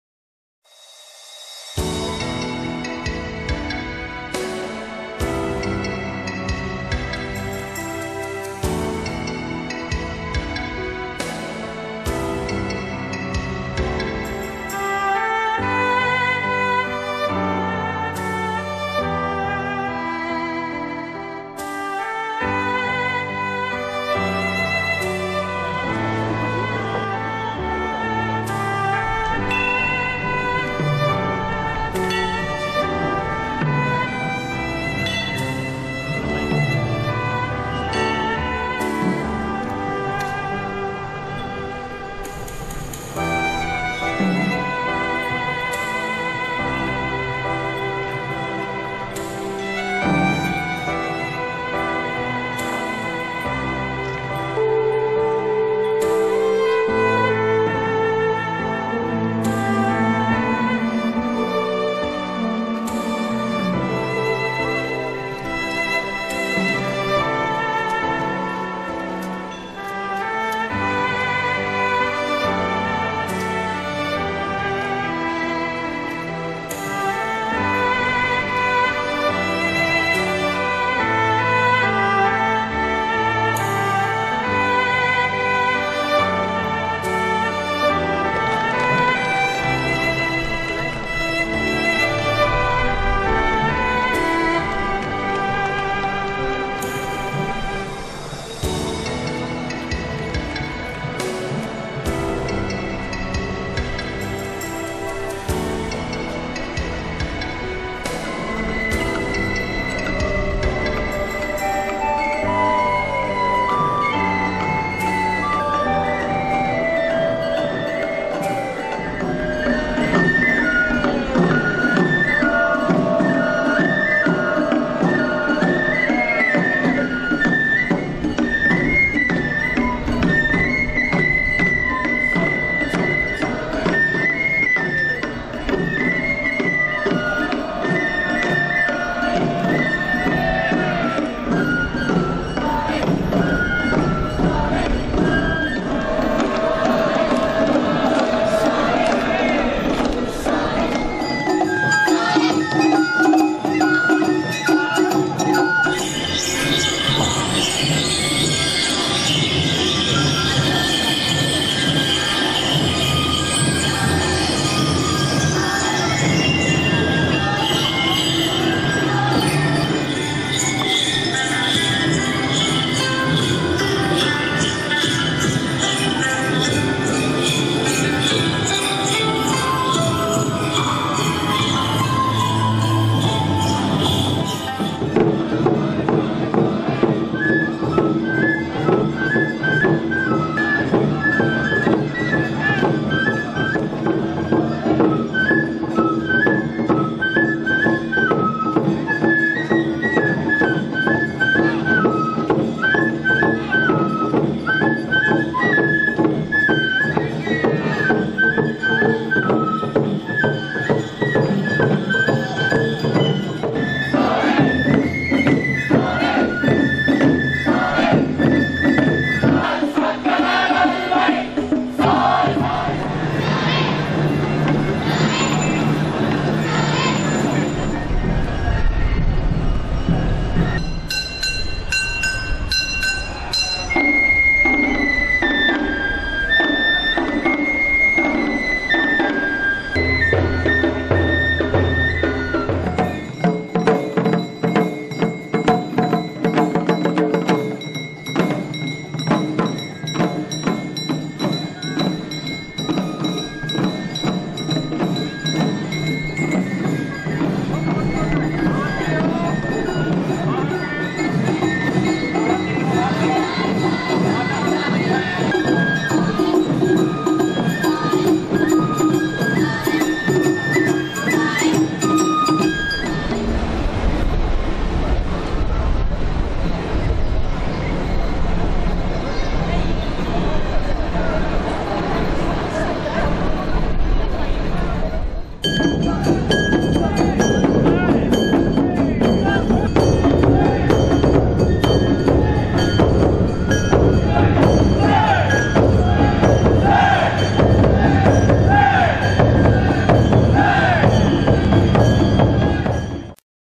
【仙台あおばまつり】2012青葉まつり仙台武将隊・和太鼓 （宮城県仙台市）
平成２４年５月２０日ＨＤカメラ撮影。
初めて見ました。昨年は被災したため自粛し、中止しましたので２年ぶりに復活した仙台・青葉まつりでした。伊達政宗を祝う行事でしょうか。大名行列などさすが大都市仙台市です。多くの仙台市民が参加しています。観客もたくさん見に来て溢れんばかりの人出でした。オープニングはデジカメから使用しました。